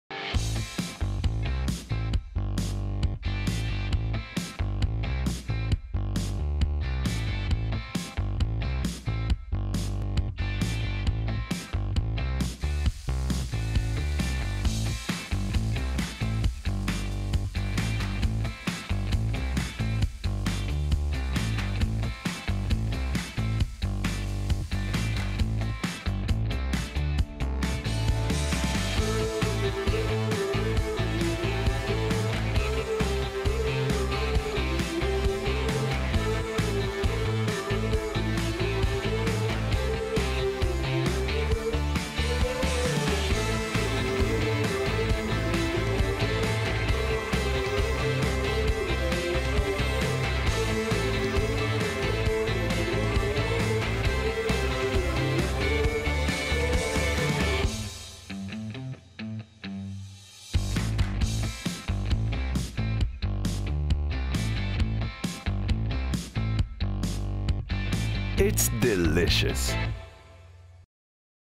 Cheesy Salmon Pie
We’ve tried to describe in one word this puff pastry pie. And we’ve come up with ‘delicious’. It’s flaky and mildly crunchy. It's cheesy and it is savory with all the veggie flavors blending irresistibly with that of the smoked salmon. Now you try it!